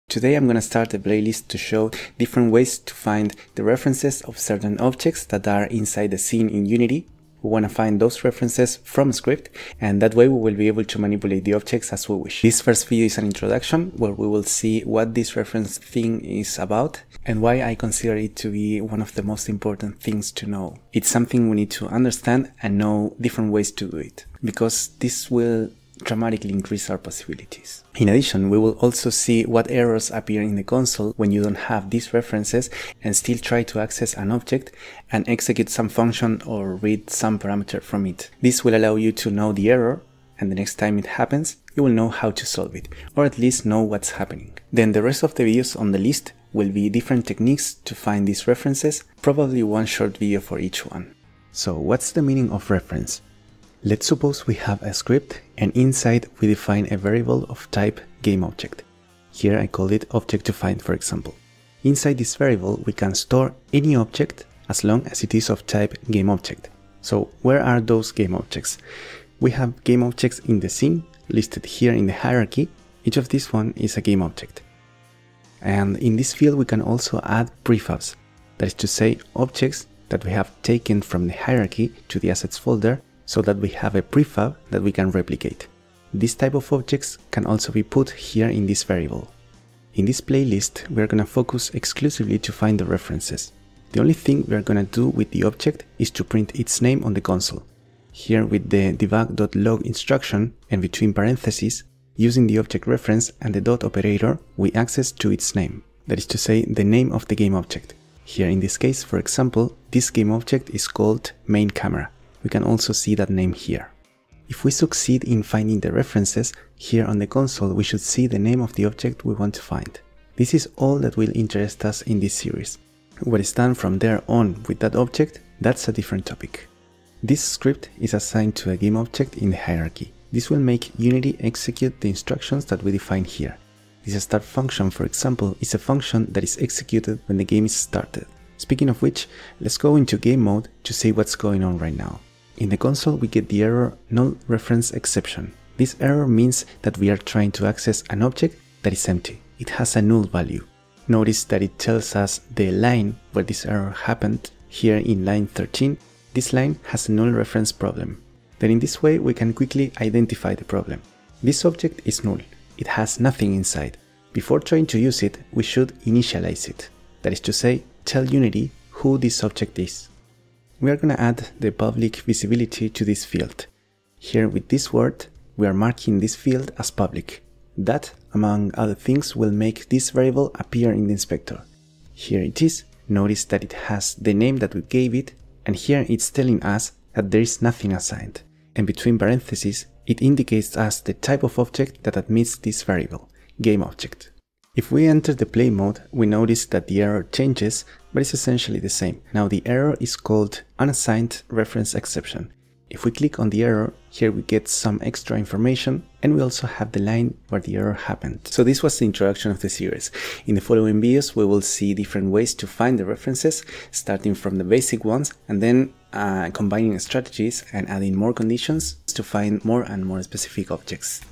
How to use GAMEOBJECTS and COMPONENTES from the Scene in scripts in Unity, Null reference exception
Practically anything we want to do in Unity will require a script that is responsible for performing these tasks, these scripts will generally work with the objects in the scene and its components, for example if we want to control a ship we will read the motion inputs and apply the movement to the object of the ship with a certain speed to make this object move across the screen.
This video is the introduction to a series that focuses on obtaining those objects and components of the scene, how we can do to work with them from a Script, this is known as REFERENCE in programming, having an object or component stored in a variable allows us to access their information and use their functions, if they are public, this is done by applying the DOT OPERATOR on the variable that has stored the object or the specific component that we want to read or execute function.

References in programming is a very important concept in object oriented programming, in this series we are going to see different methods to achieve the same thing, to have the reference of a GameObject or scene component in a variable defined in a script.
In this video we also see what console errors can appear when we do not have the object reference but we still try to use it, the NullReferenceException and UnassignedReferenceException errors.

*LINKS*